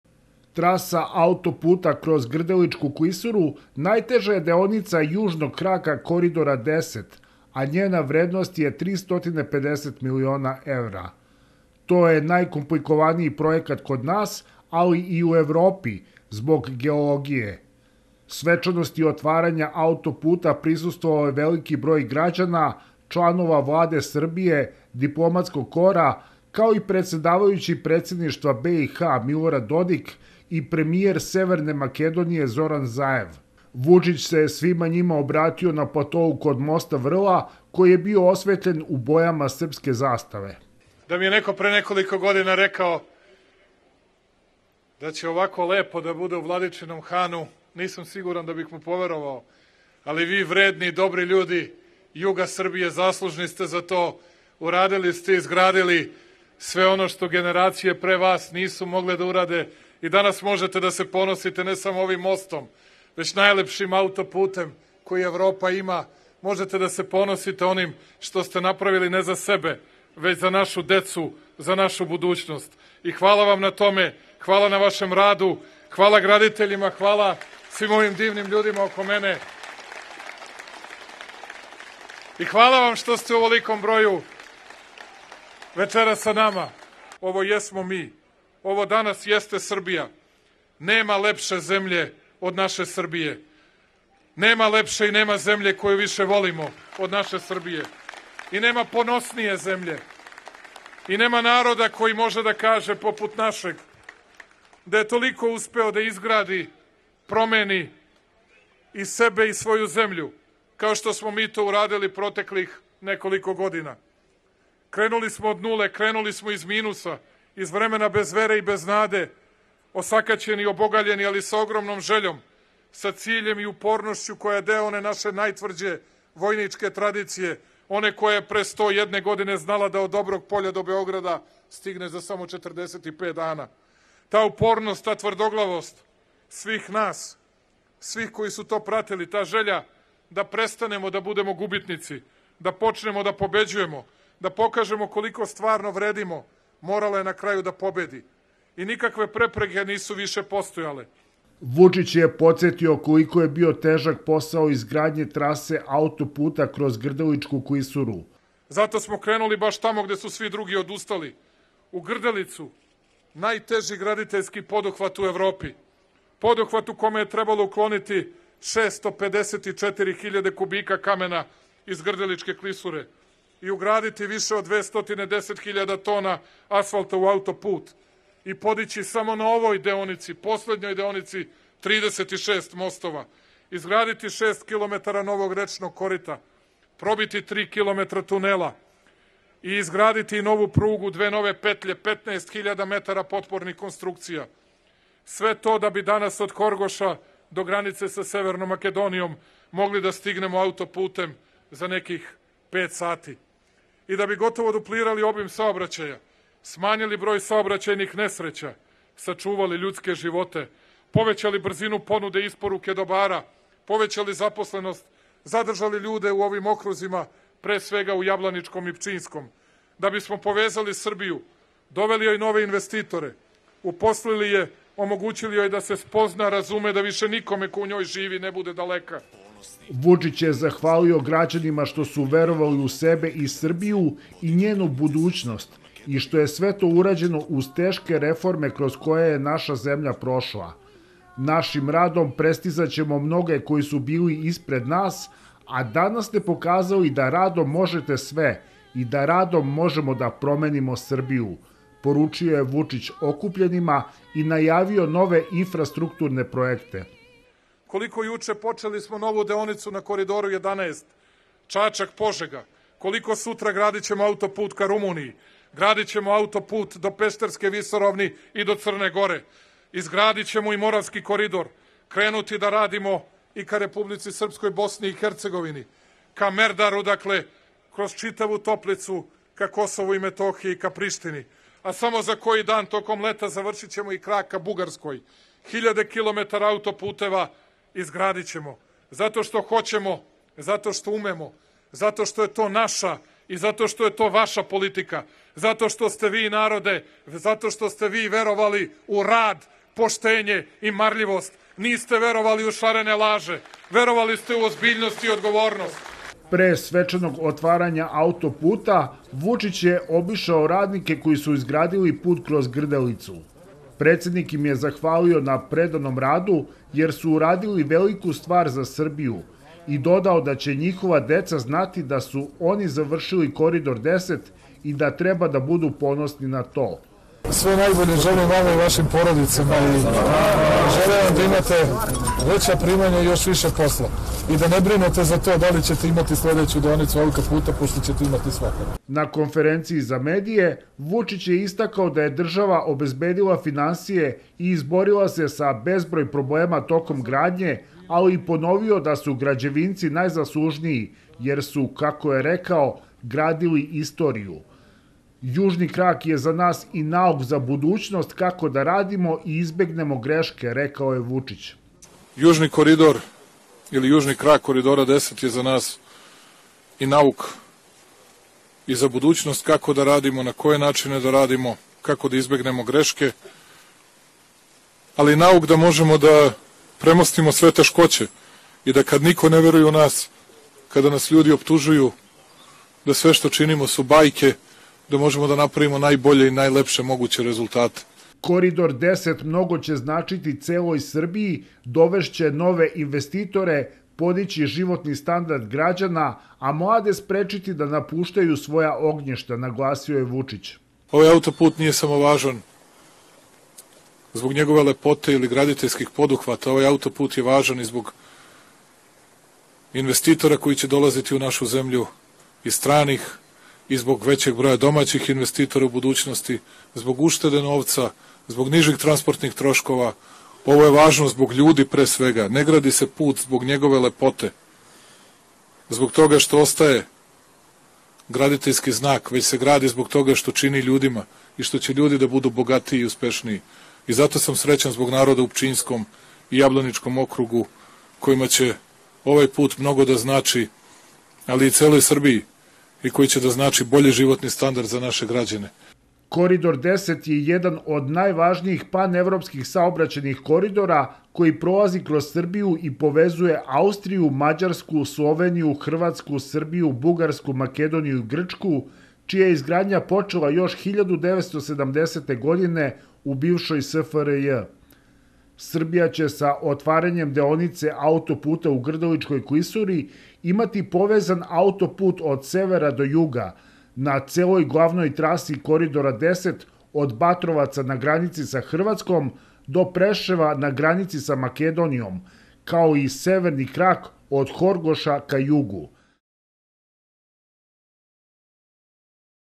VTR Svecano Otvaranje koridor 10
"Kiptim od sreća, radosti i ponosa, za ovako nešto veliko, moćno i lepo, što nadilazi naše snove", kazao je predsednik Vučić